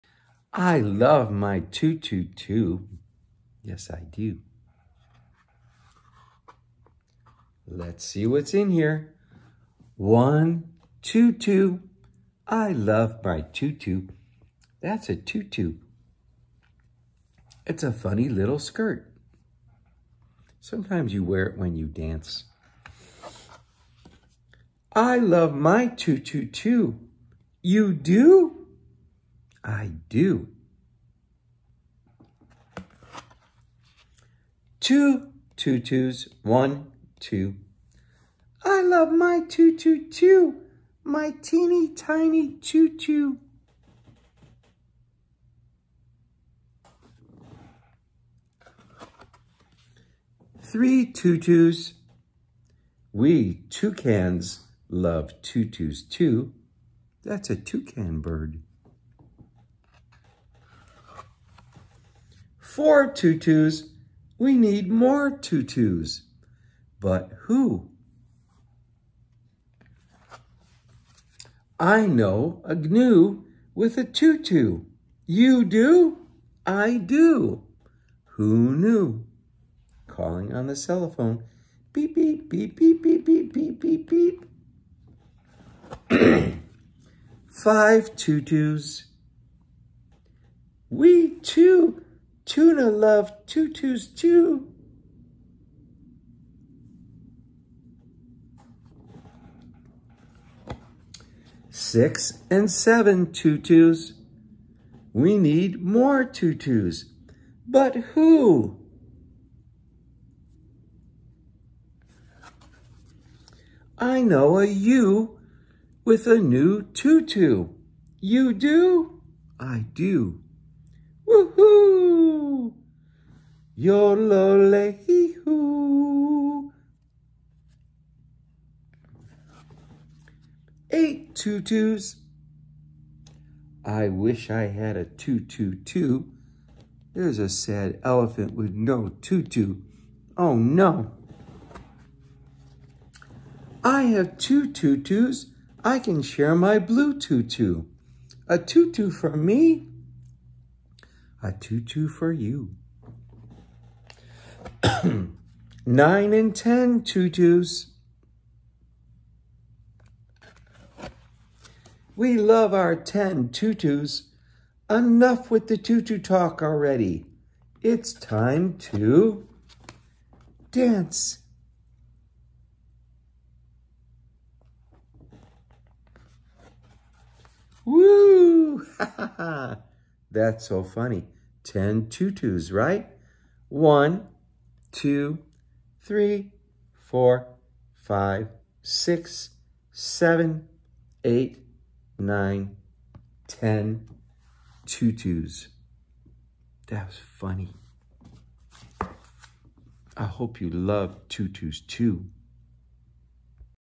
Papa Marsh Reads, "I Love My Tutu Too"
By Ross Burach.  A Never Bored Book
What's more irresistible than a pink tutu? Dancing in a pink tutu, of course, and inviting friends to join you!
* "A rollicking tutu lovefest." -- Kirkus Reviews, starred review* "Irresistible... Wacky and wonderful." -- School Library Journal, starred review
In this jubilant rhyming romp, a penguin, a bear, a toucan, and even a gnu (who knew?) are all wearing their tutus today. As a parade of other curious critters join the fun -- I know a ewe with a new tutu. You do? I do. Woo-hoo! -- the number of tutus grows, until they reach 10 tutus. Then it's time to dance!
Abundant alliteration and rollicking rhythms will have little ones laughing and dancing till they drop -- and joyfully learning to count along the way.